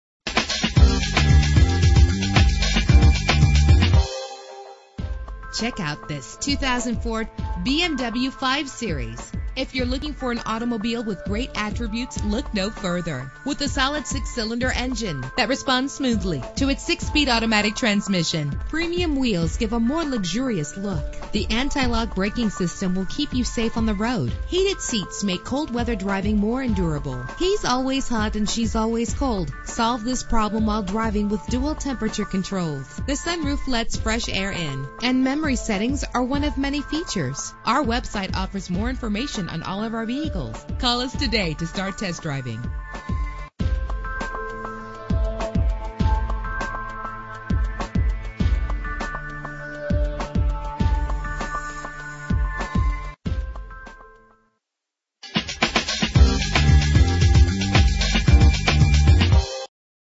2004 BMW 5 Series - Bloomington MN
Year: 2004
Make: BMW
Model: 5 Series
Trim: 530i
Engine: 3.0 liter inline 6 cylinder 24 valve
Transmission: 6-Speed Automatic
Color: Gray
Mileage: 47830
Address:
1300 American Blvd West